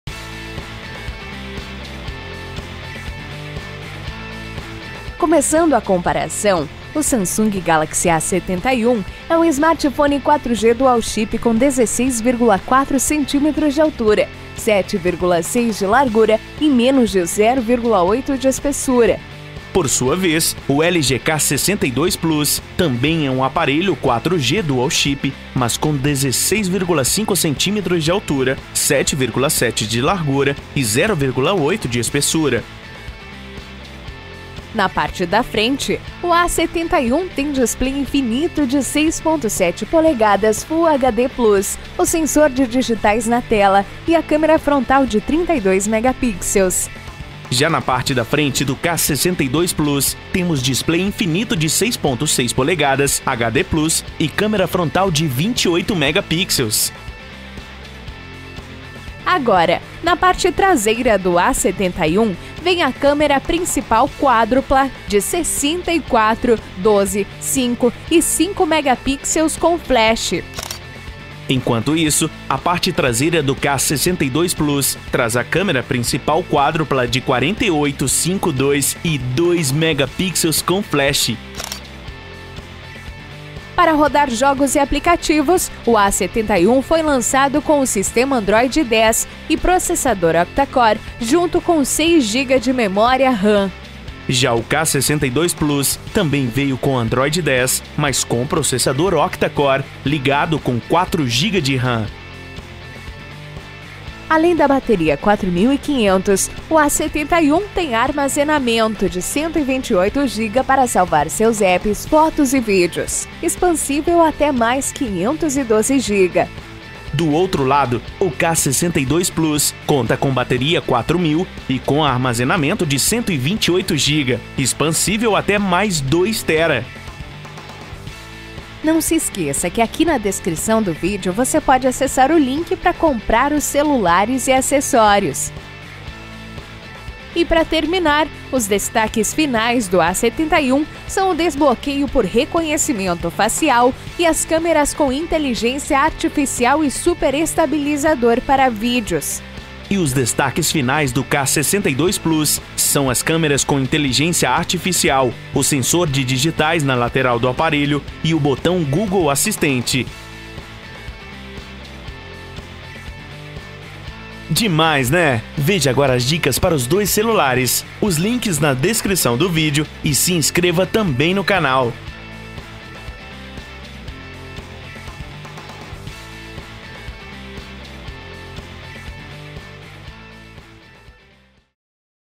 A71 vs LG K62 Plus (Comparativo)
Samsung e LG fisicamente parecidos no comparativo entre a ficha técnica do Galaxy A71 e do LG K62 Plus, + uma comparação de tamanho exclusiva (3D), e muito mais.

Links para comprar:

(Galaxy A71)

(LG K62 Plus)

Pergunta:
Qual celular lançado recentemente tem as maiores bordas na opinião de vocês?